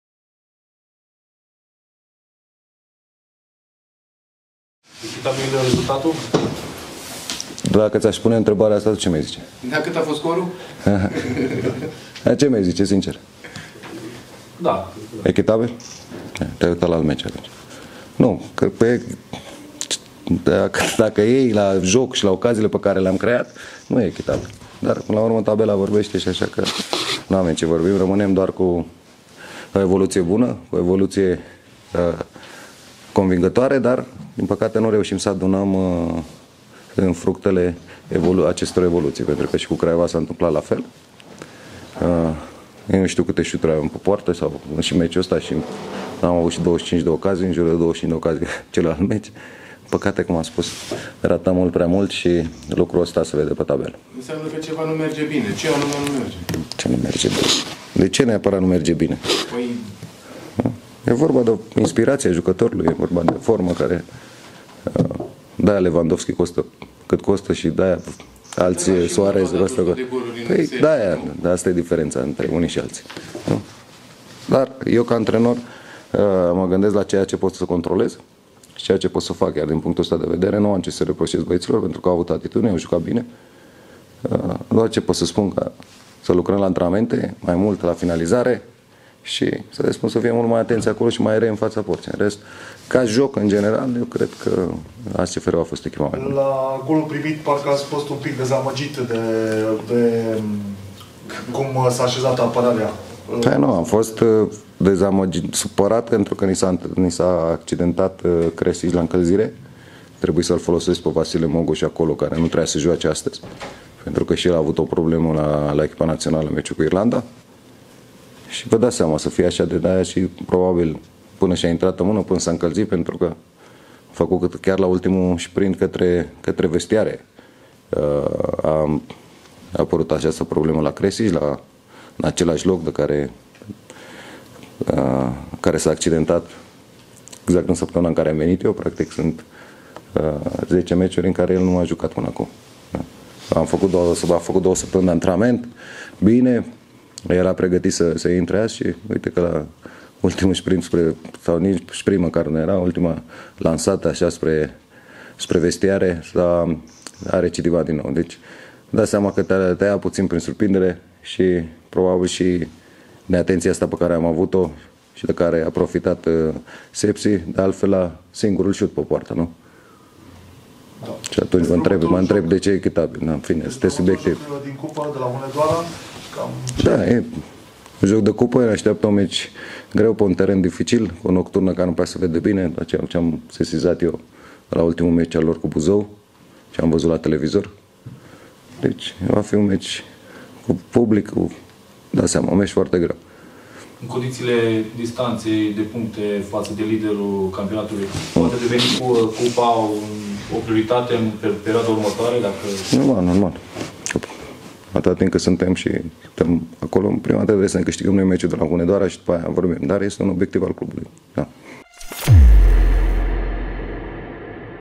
🎥 Sepsi - CFR Cluj | Mutu: “Ratăm prea mult și asta se vede pe tabelă”
🎤 Concluziile antrenorului nostru, Adrian Mutu, după meciul din etapa a doua a play-off-ului, cu Sepsi OSK ⚽